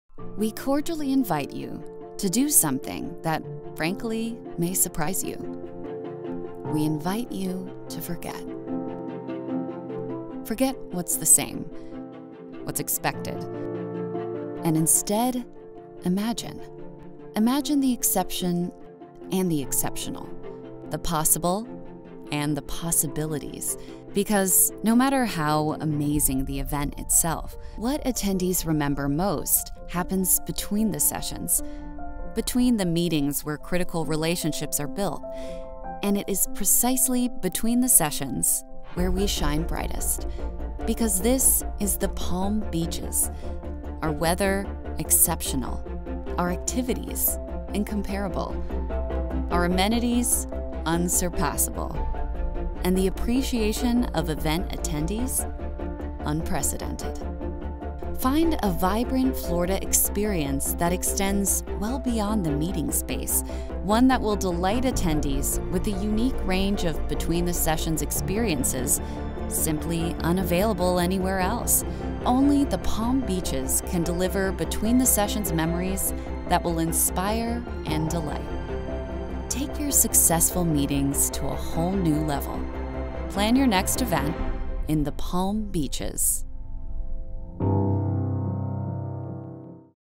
Set Meetings Apart Between-The-Sessions in The Palm Beaches
Unforgettable Meetings & Between-the-Sessions Experiences
This is where meeting spaces of every size boasting the latest technology share the stage with vibrant communities and breathtaking natural beauty. Beyond the insights and connections gained within the boardroom, The Palm Beaches take care of the rest with inspiring venues, unique opportunities to connect, and Between-The-Sessions experiences that set meetings apart.

Plan in a destination that knows meetings — and understands how to make yours stand out. Our award-winning team and community partners collaborate with you in planning from day one. Our world-class hotels and meetings venues shine against the backdrop of a destination that’s unmatched in natural beauty. Our outdoor spaces, dining and entertainment options, cultural institutions, attractions and commitment to service will elevate your event and energize attendees.

The Palm Beaches are a hub of bustling industries, including agribusiness, life sciences, finance and technology. Connect your attendees with innovators and thought leaders who will bring expertise and star power to your event.